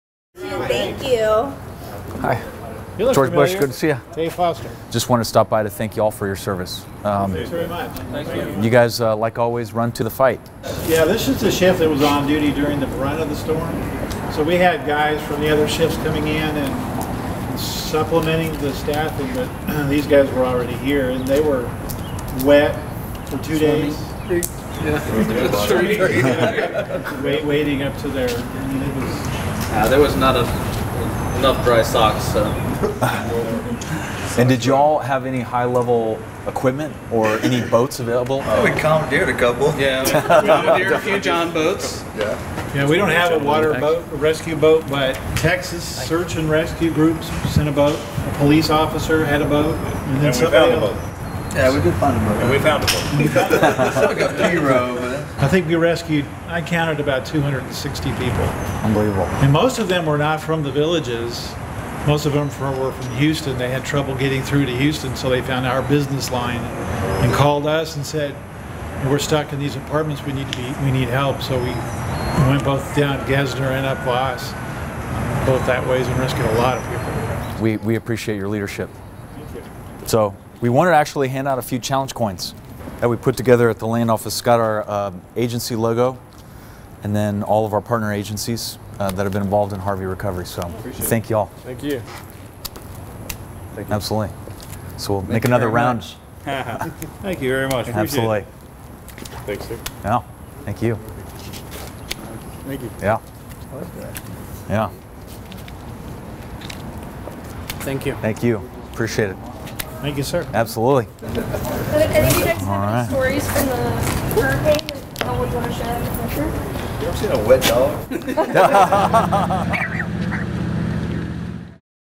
Cmr. George P. Bush Presents Challenge Coins to Village Firefighters
February 21, 2018 - Today Texas Land Commissioner George P. Bush awarded the fire fighters of the Village Fire Department in Houston with commemorative Texas Rebuilds Challenge Coins for their heroic efforts as first responders during Hurricane Harvey.